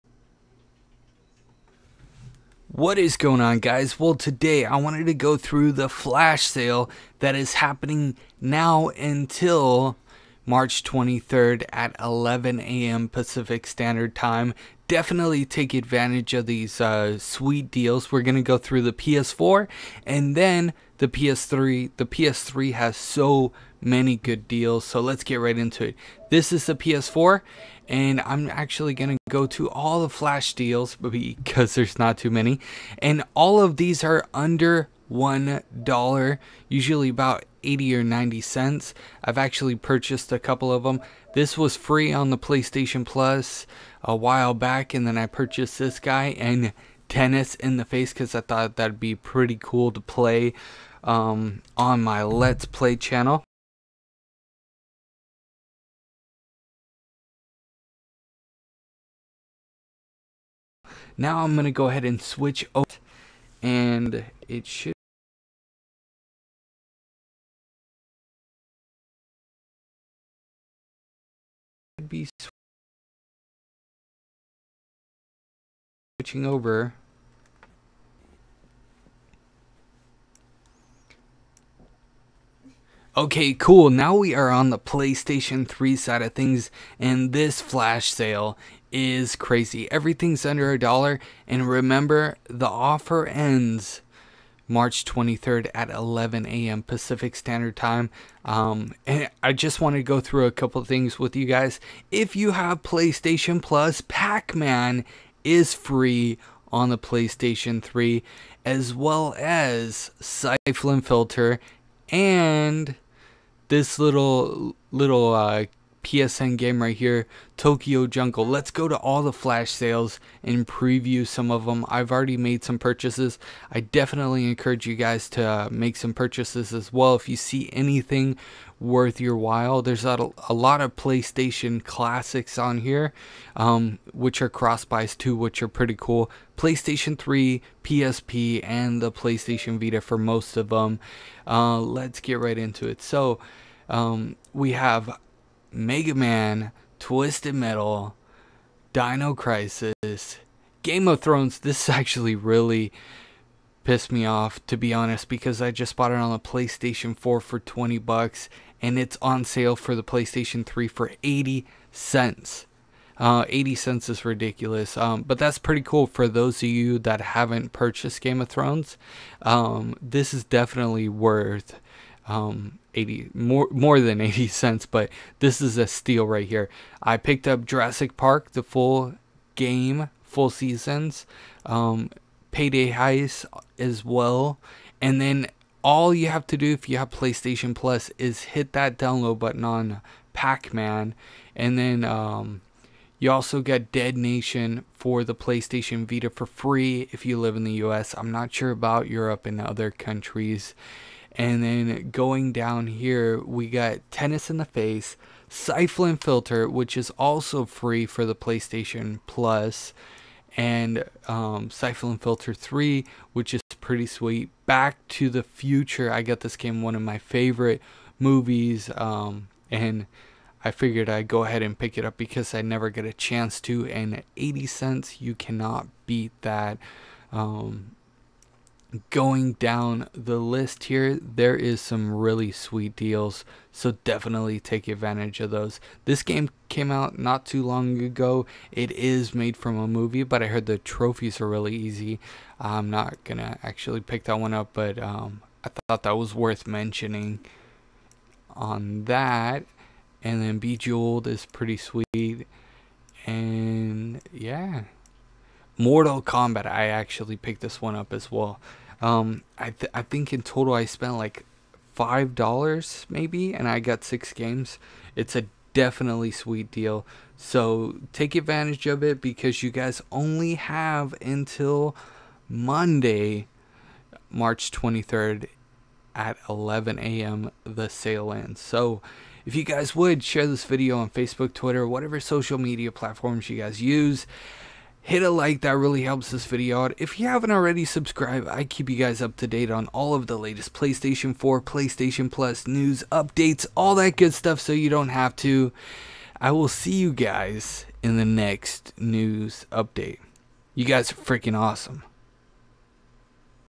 PSN FLASH SALE EXTRA FREE PS3 GAMES for PS PLUS MEMBERS
PSN FLASH SALE ALL Games under $1 PS4 PS3 VITA PSP PSN FLASH SALE EXTRA FREE GAMES for PS PLUS MEMBERS OFFER ENDS MARCH 23 2015 We should get another Flash Sale in April with PS PLUS offers but nothing like this sale so take advantage while you still can!!

PSN: MonkeyFlop

Game On!!
Family Friendly